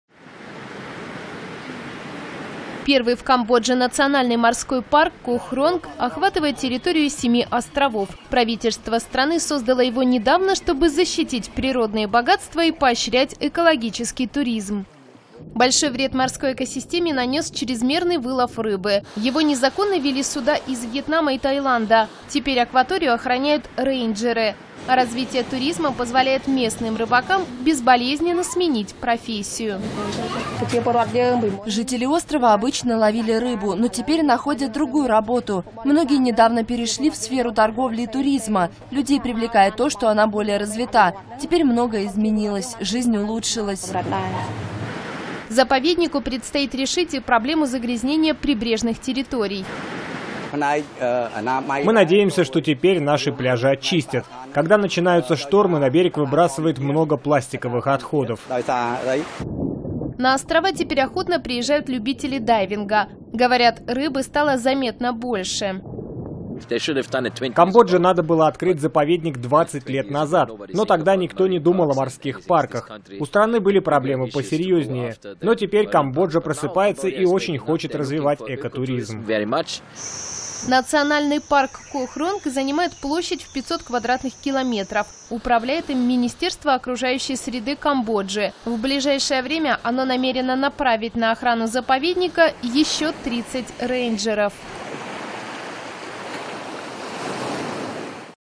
Первый морской заповедник в Камбодже защитит экосистему
Камбоджа надеется, что создание нового заповедника Кох-Ронг поможет защитить экосистему её островов. Это первый национальный морской парк в стране.